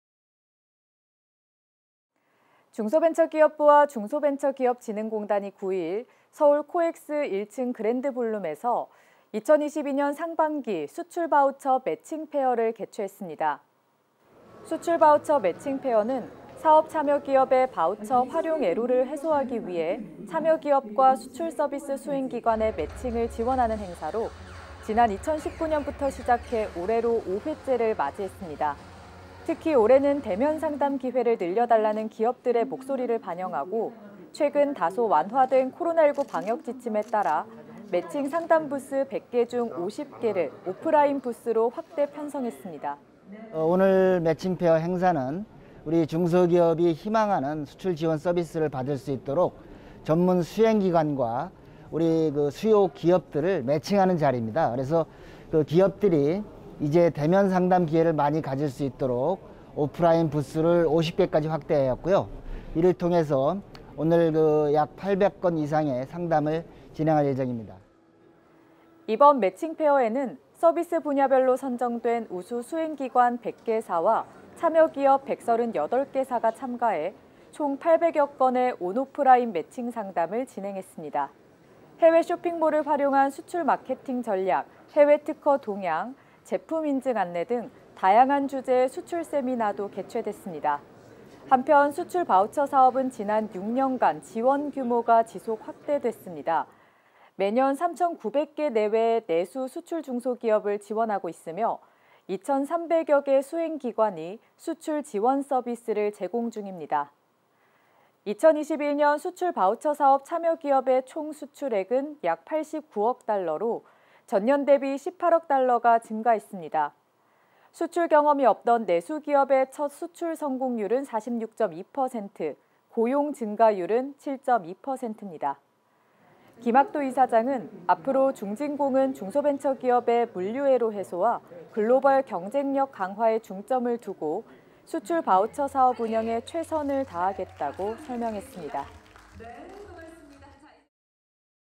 [yestv뉴스] 2022년 상반기 수출바우처 매칭페어
중소벤처기업부와 중소벤처기업진흥공단이 9일 서울 코엑스 1층 그랜드볼룸에서 '2022년 상반기 수출바우처 매칭페어'를 개최했습니다.

수출바우처 매칭페어는 사업 참여기업의 바우처 활용 애로를 해소하기 위해 참여기업과 수출 서비스 수행기관의 매칭을 지원하는 행사로 지난 2019년부터 시작해 올해로 5회째를 맞았습니다.

특히 올해는 대면상담 기회를 늘려달라는 기업들의 목소리를 반영해, 최근 다소 완화된 코로나19 방역지침에 따라 매칭 상담부스 100개 중 50개를 오프라인 부스로 확대 편성했습니다.

이번 매칭페어에는 서비스 분야별로 선정된 우수 수행기관 100개사와 참여기업 138개사가 참가해 총 800여건의 온·오프라인 매칭상담을 진행했습니다.

해외 쇼핑몰을 활용한 수출마케팅 전략, 해외특허동향·제품인증 안내 등 다양한 주제의 수출세미나도 열렸습니다.

한편 수출바우처사업은 지난 6년간 지원규모가 지속 확대됐습니다. 매년 3900개 내외의 내수·수출 중소기업을 지원하고 있으며, 2300여개 수행기관이 수출 지원 서비스를 제공 중입니다.

2021년 수출바우처사업 참여기업의 총 수출액은 약 89억 달러로, 전년 대비 18억 달러가 증가했습니다. 수출경험이 없던 내수기업의 첫 수출성공률은 46.2%, 고용증가율은 7.2%입니다.

김학도 이사장은 "앞으로 중진공은 중소벤처기업의 물류애로 해소와 글로벌 경쟁력 강화에 중점을 두고 수출바우처사업 운영에 최선을 다하겠다"고 말했습니다.